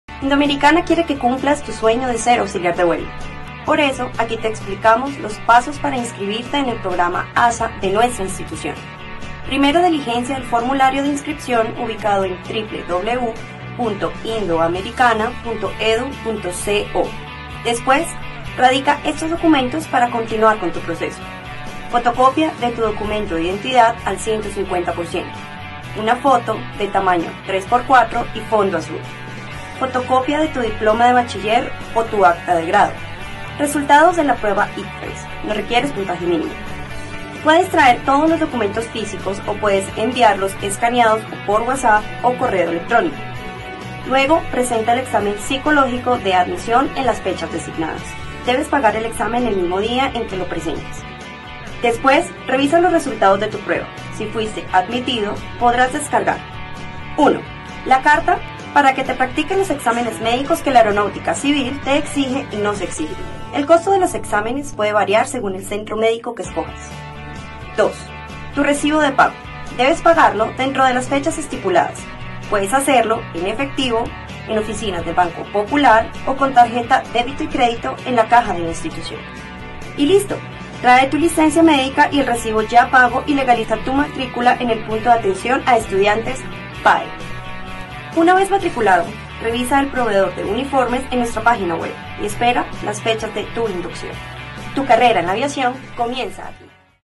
Proceso de inscripcion ASA
Conoce el proceso de inscripción a nuestro programa de Auxiliar de vuelo